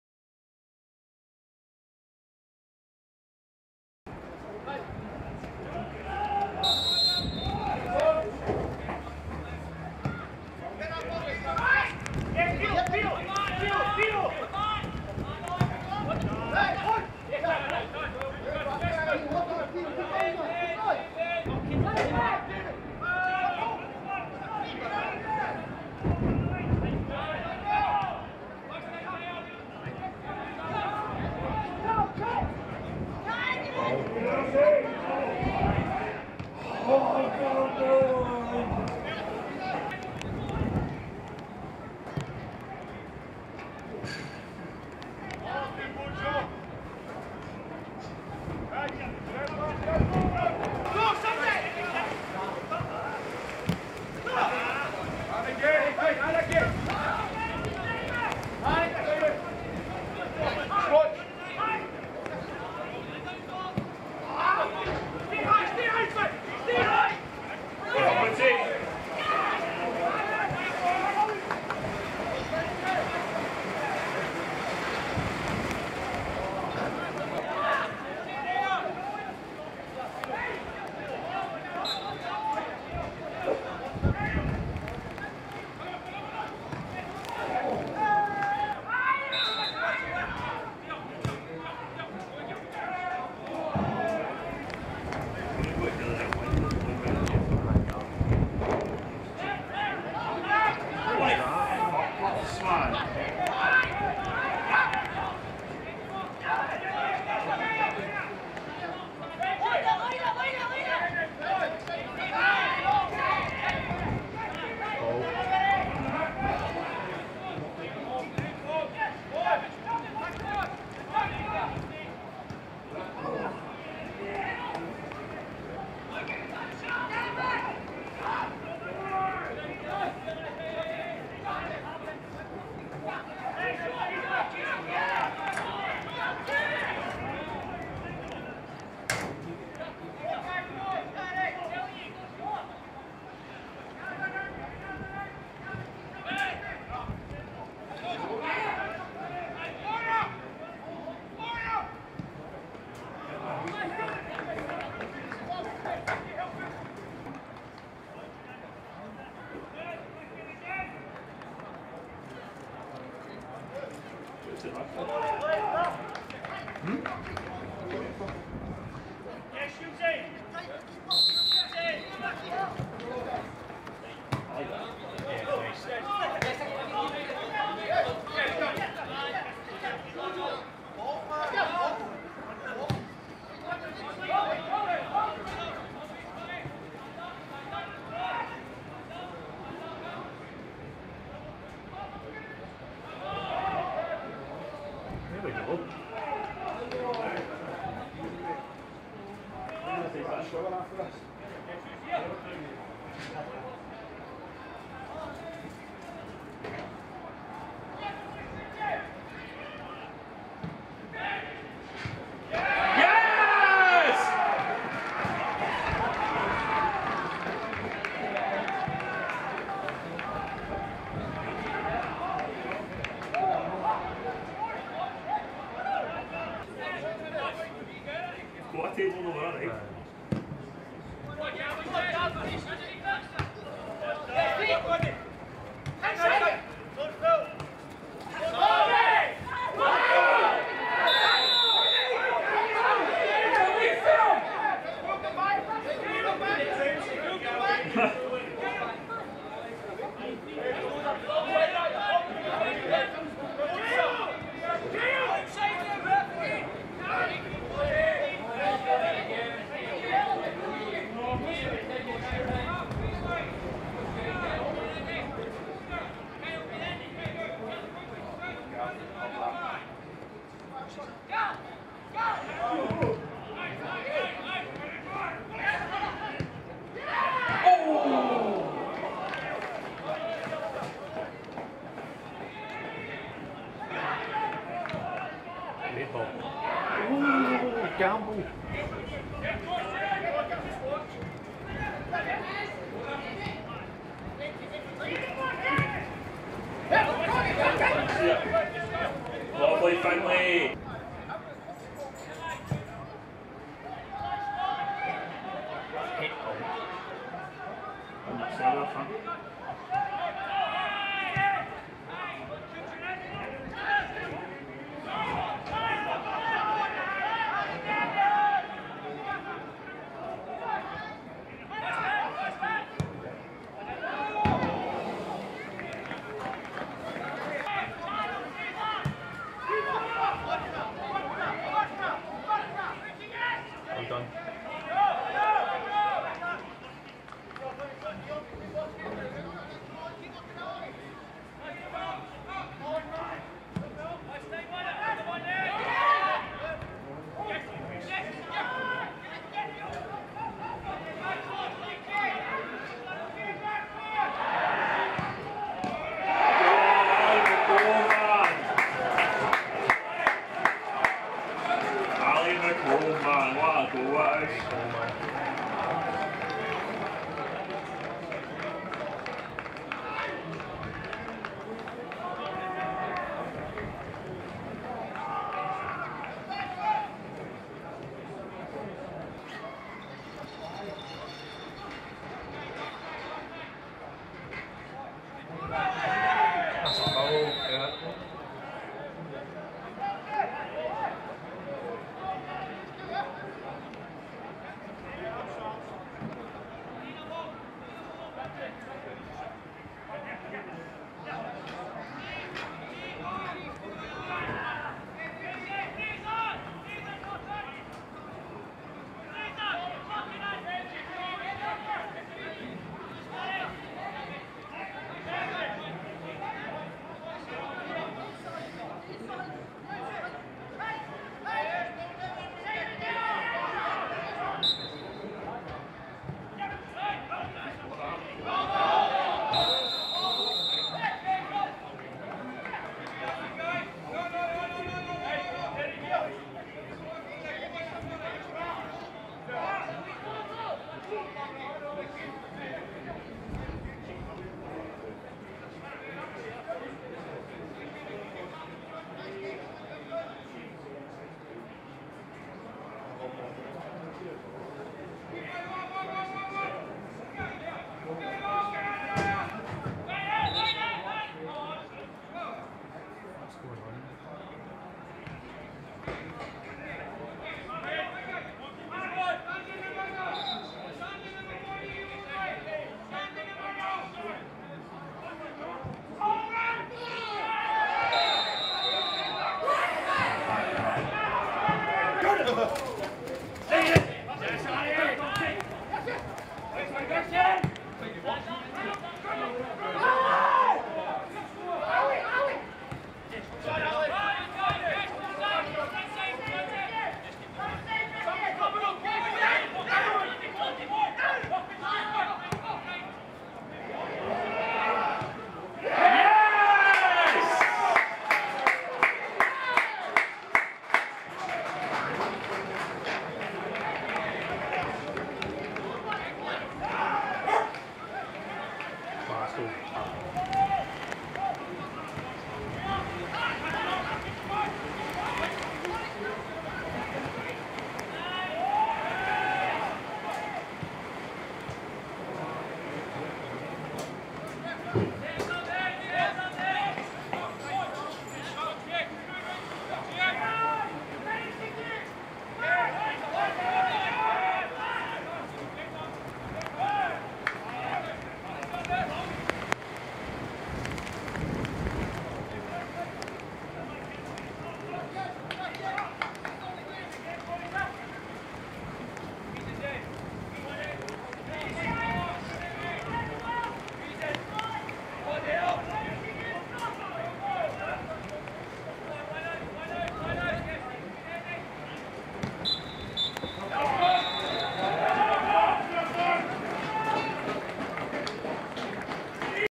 HIGHLIGHTS: Kilwinning Rangers 3-0 Rutherglen Glencairn
All the action from our 3-0 win over Rutherglen. Goals from David Syme, Ally McColm and Thomas Collins